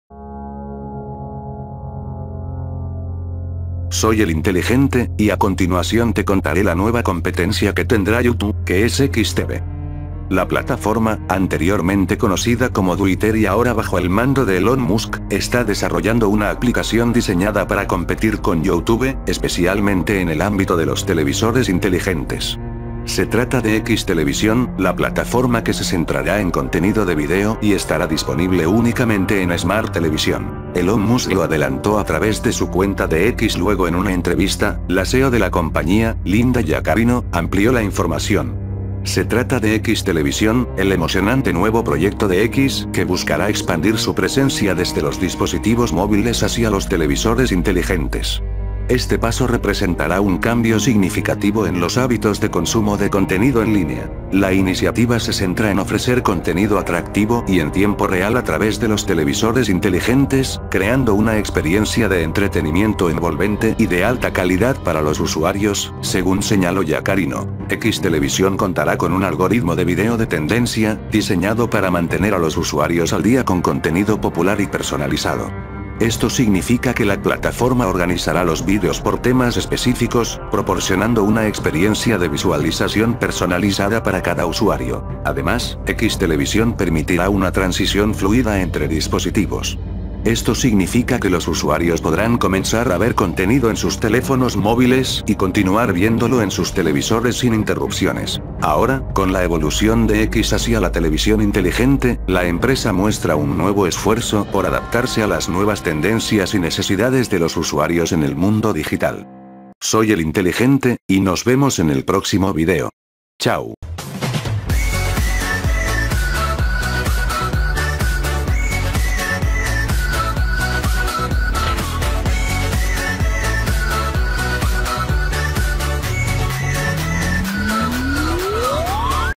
Elon Musk crea una aplicación para ver videos en el televisor • El Inteligente
El dueño de la red de microblogging quiere crear un centro único en el mundo de audio, video, mensajes y hasta pagos. La red social X, antes conocida como Twitter, está tomando un nuevo rumbo estratégico que la coloca directamente en competencia con plataformas populares del streaming de vídeo como YouTube.

El Inteligente ©